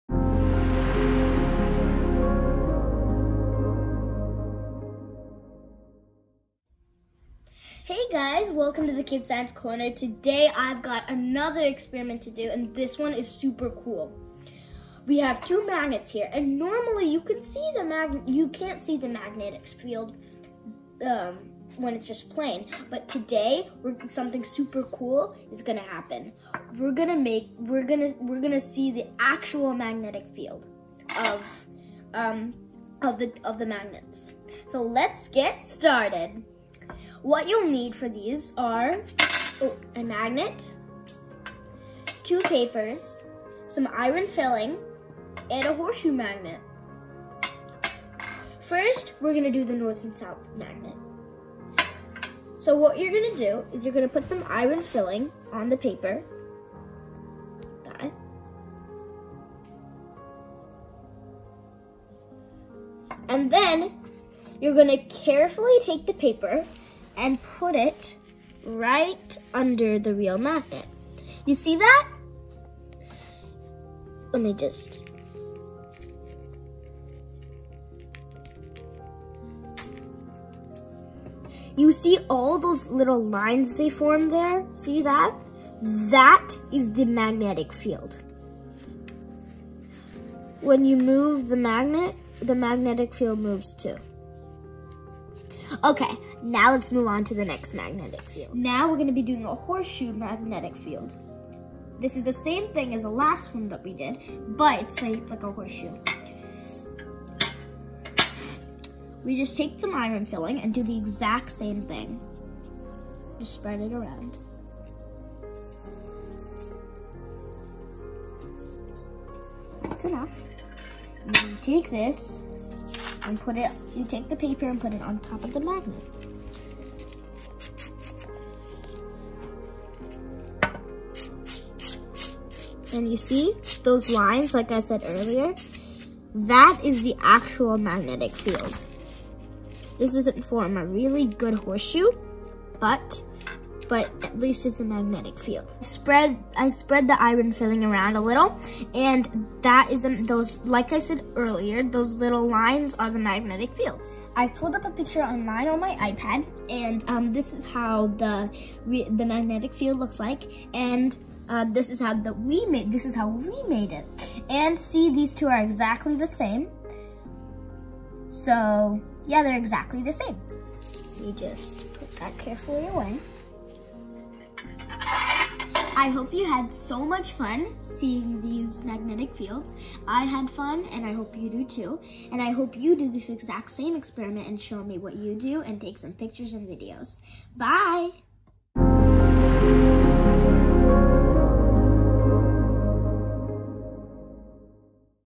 Magical Magnetic Field
In this experiment you will actually see a magnetic field!!